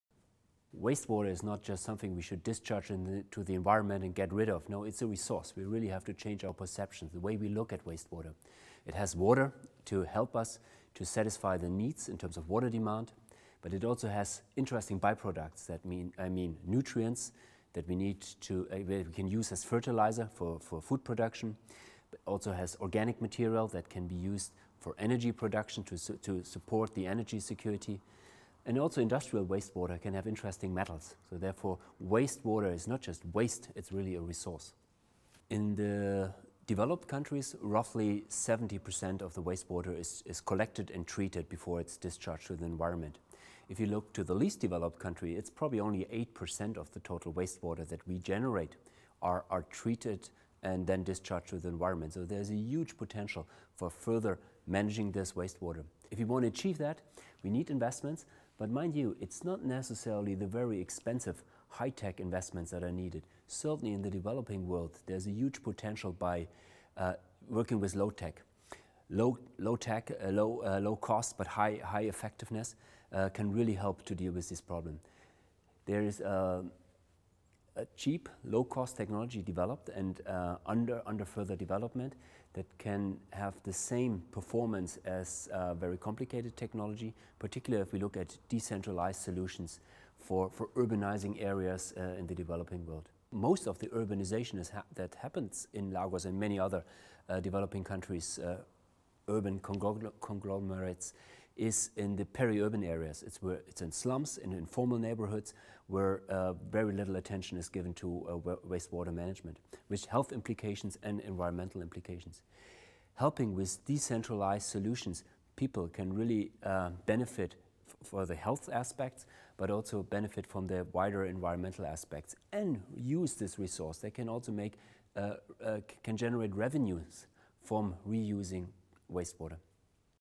Wastewater: the Untapped Resource
What if we were to consider the vast quantities of domestic, agricultural and industrial wastewater discharged into the environment everyday as a valuable resource rather than costly problem? This is the paradigm shift advocated in this year’s United Nations World Water Development Report, “Wastewater: the Untapped Resource ». It argues that once treated, wastewater could prove invaluable in meeting the growing demand for freshwater and other raw materials.
Stefan Uhlenbrook, Coordinator of the World Water Assessment Programme, explains the main conlusions of the report.